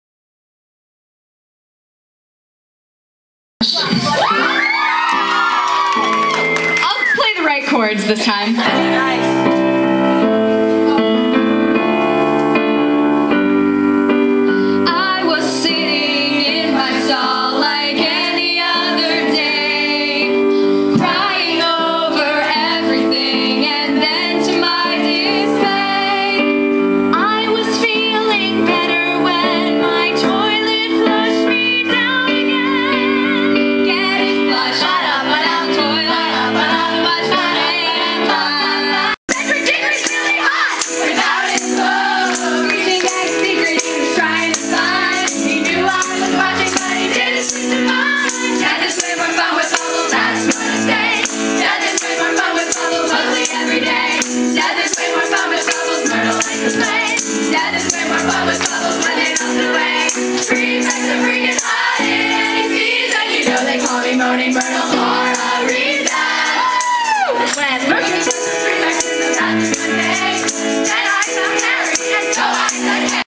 The Moaning Myrtles at the Philly Wrock Show
A bit of The Moaning Myrtles from their performance at the Philly Wrock Show on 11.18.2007. Enjoy the wrock.